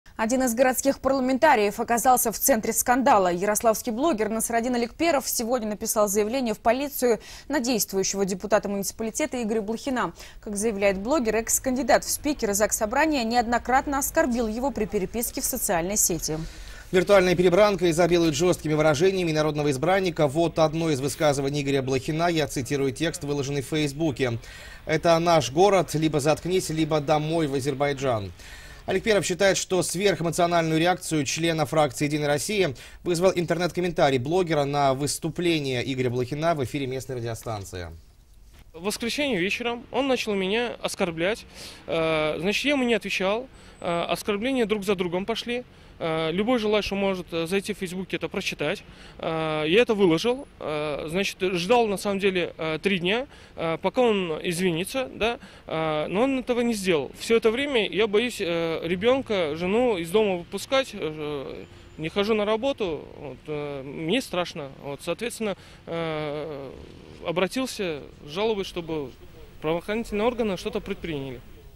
Ярославский парламентарий оказался в центре скандала
Ярославский блогер Насраддин Алекперов сегодня написал заявление в полицию на действующего депутата муниципалитета Игоря Блохина.